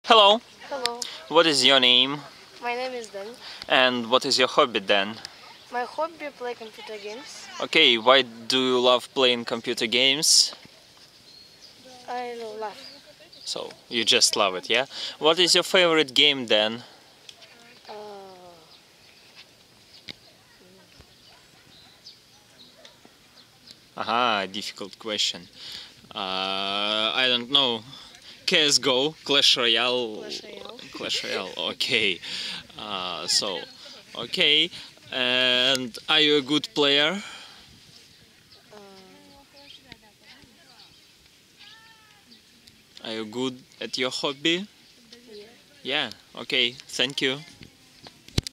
Dan hobby interview
Students gave interviews about their hobbies and free time activities